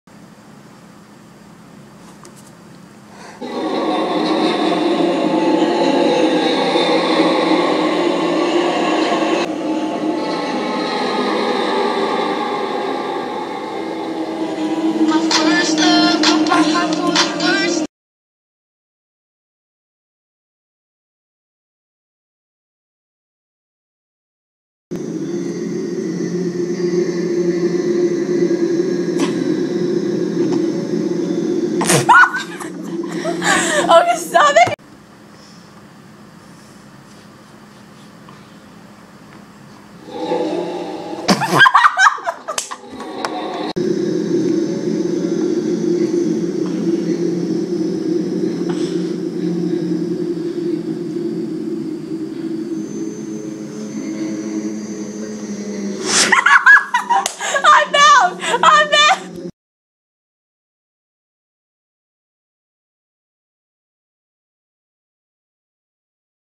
What Will Happen?
Evangeline & I Wait For Someone To Txt Us An We Hear Scary Noises As We Wait, We Wish We Knew What Bloopers At End ! Enjoy